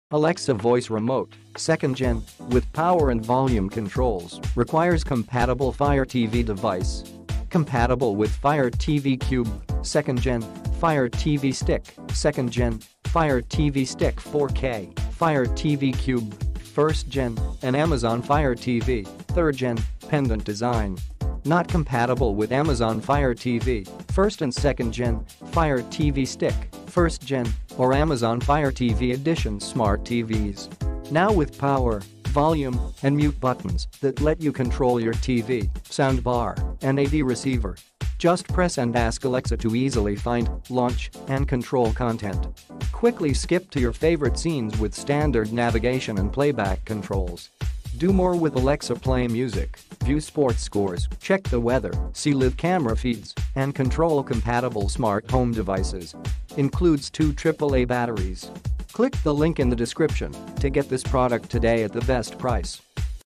Alexa Voice Remote (2nd Gen) with power and volume controls – requires compatible Fire TV device
Click here for best price :

Compatible with Fire TV Stick Lite, Fire TV Stick 2020 Release, Fire TV Stick (2nd Gen), Fire TV Stick 4K, Fire TV Cube (2nd Gen), Fire TV Cube (1st Gen), and Amazon Fire TV (3rd Gen, Pendant Design). Not compatible with Amazon Fire TV (1st and 2nd Gen), Fire TV Stick (1st Gen), or Amazon Fire TV Edition smart TVs.
Now with power, volume, and mute buttons that let you control your TV, sound bar, and A/V receiver.
Just press and ask Alexa to easily find, launch, and control content.
Quickly skip to your favorite scenes with standard navigation and playback controls.
Do more with Alexa—play music, view sports scores, check the weather, see live camera feeds, and control compatible smart home devices.
Includes 2 AAA batteries.

Best Price